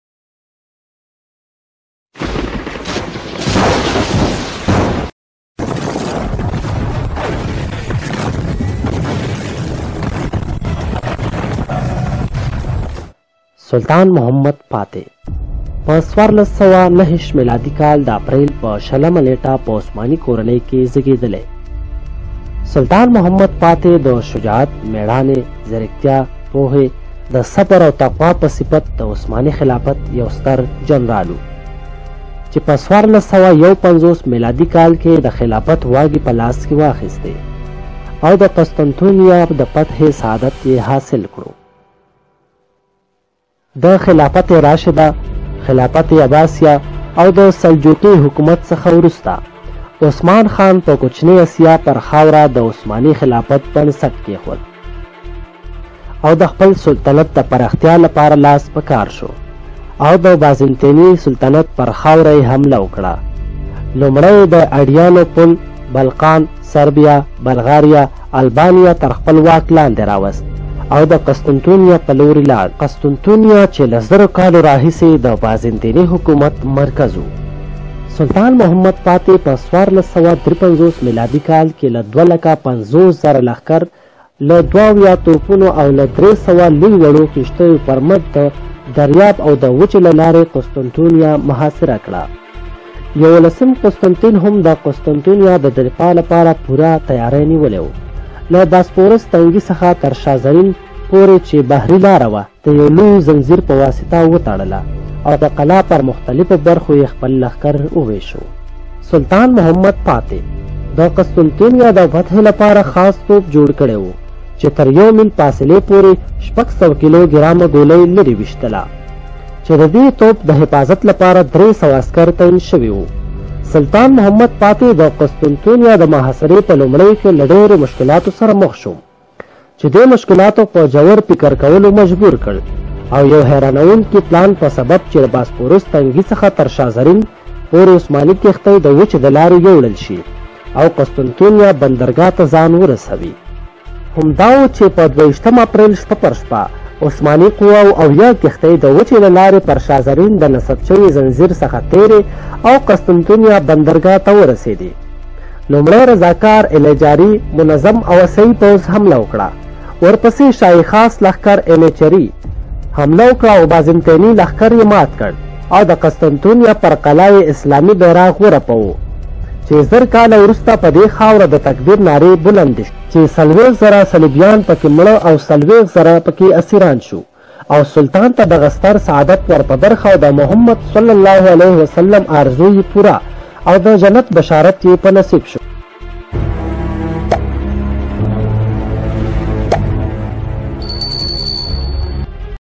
Muhammad Sani life story | 2nd Mohammad | Turkey 2023 | UK Vines Kabul
Story of Sultan Mohammad fatih and what will happen in Turkey at 2023